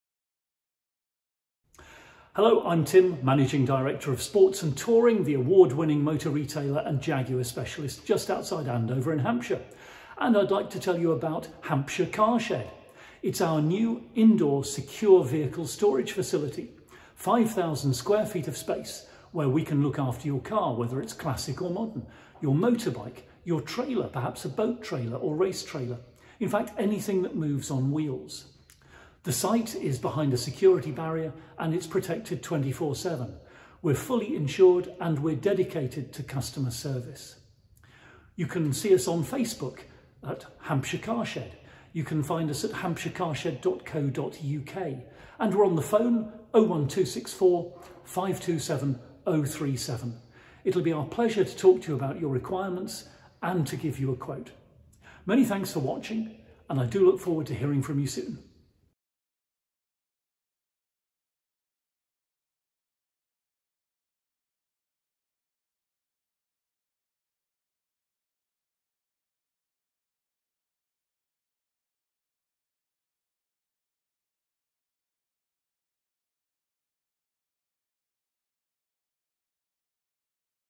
HCS intro
Introducing Hampshire Car Shed, the secure vehicle storage facility from Sports & Touring!